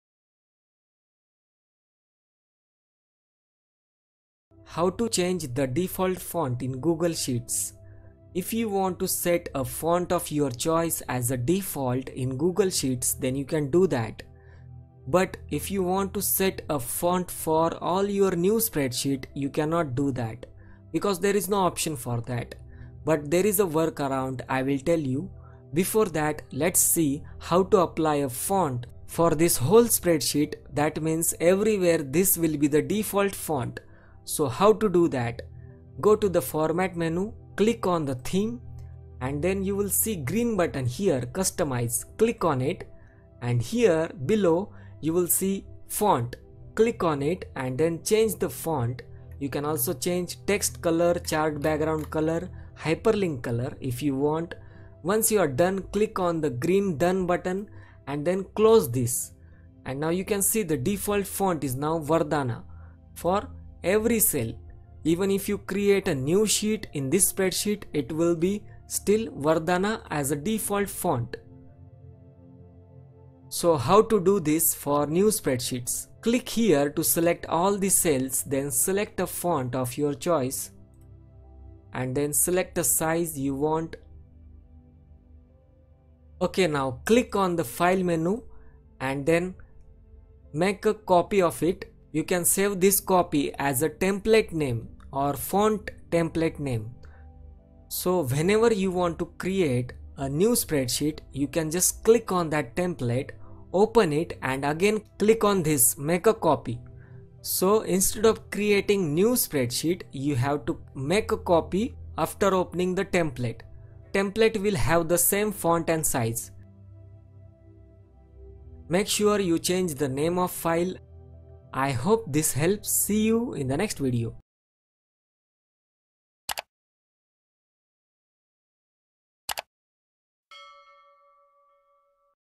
Set a Default Font in Google Sheets
0:00 Intro
0:35 Change Theme
1:12 Create a template

Change default font in google sheets using theme option and template trick.

GoogleSheets​ is a comprehensive video series of Google Sheets tutorials by The Akshay.